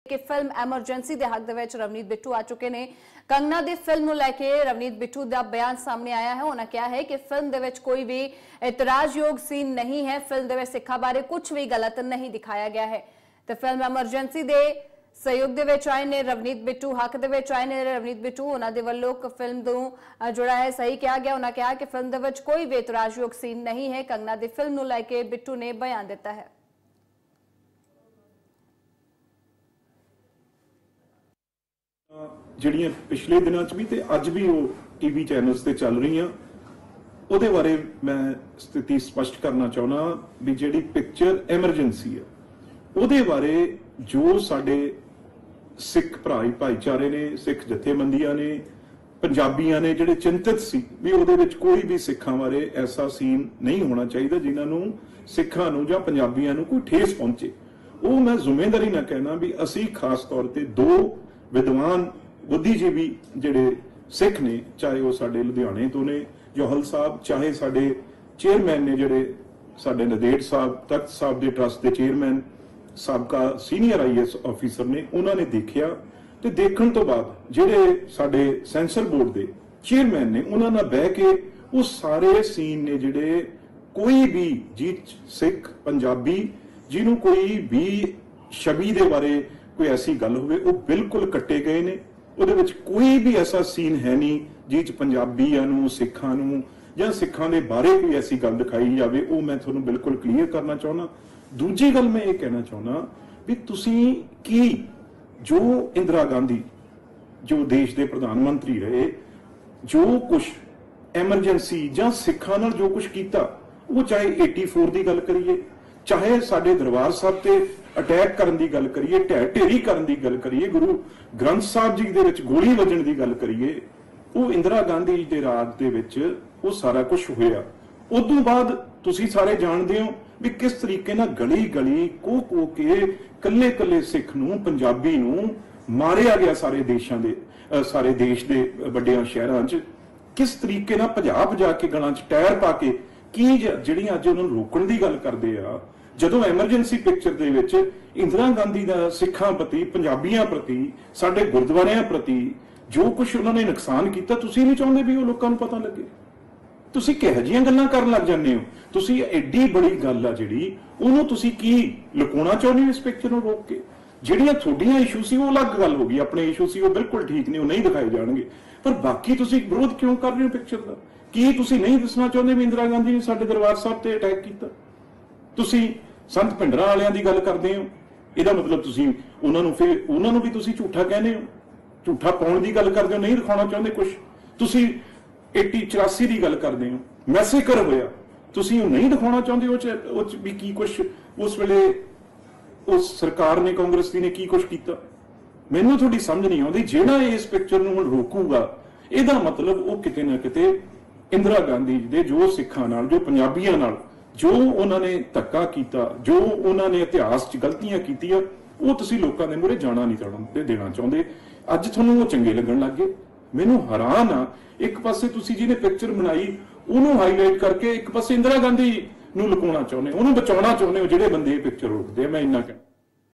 Emergency | Kangana Ranaut | Ravneet Bittu | ਫਿਲਮ 'ਐਮਰਜੈਂਸੀ' ਦੇ ਹੱਕ 'ਚ ਰਵਨੀਤ ਬਿੱਟੂ INDIA NEWS PUNJAB
ਫਿਲਮ 'ਐਮਰਜੈਂਸੀ' ਦੇ ਹੱਕ 'ਚ ਰਵਨੀਤ ਬਿੱਟੂ ਕੰਗਨਾ ਦੀ ਫਿਲਮ ਨੂੰ ਲੈ ਕੇ ਬਿੱਟੂ ਦਾ ਬਿਆਨ ਫਿਲਮ 'ਚ ਕੋਈ ਵੀ ਇਤਰਾਜ਼ਯੋਗ ਸੀਨ ਨਹੀਂ-ਬਿੱਟੂ ਫਿਲਮ 'ਚ ਸਿੱਖਾਂ ਬਾਰੇ ਕੁੱਝ ਵੀ ਗਲਤ ਨਹੀਂ ਦਿਖਾਇਆ ਗਿਆ'

In favor of the film 'Emergency', Ravneet Bittu's statement about Kangana's film: There is no objectionable scene in the film - Bittu has not shown anything wrong about Sikhs in the film.